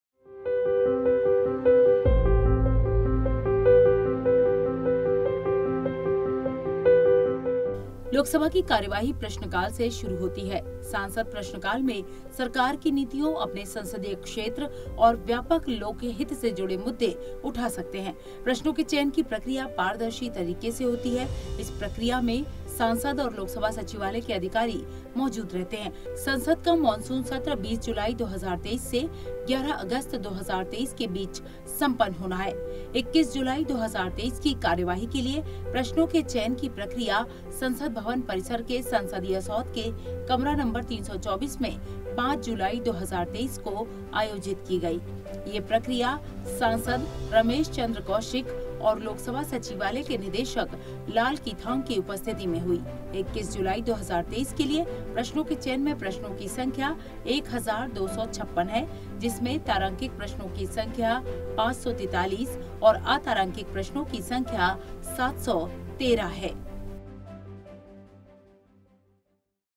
लोक सभा बैलेट प्रक्रिया प्रश्नकाल | 05 जुलाई, 2023
संसद के मॉनसून सत्र में 21 जुलाई 2023  को लोक सभा की बैठक के लिए प्रश्नों के चुनाव हेतु कंप्यूटरीकृत बैलेट की प्रक्रिया 05 जुलाई 2023 को संसदीय सौध के कक्ष संख्या 324 में  सांसद रमेश चंद्र कौशिक और लोक सभा सचिवालय के निदेशक लालकिथॉग की उपस्थिति में हुई।

Producer: Archana Mishra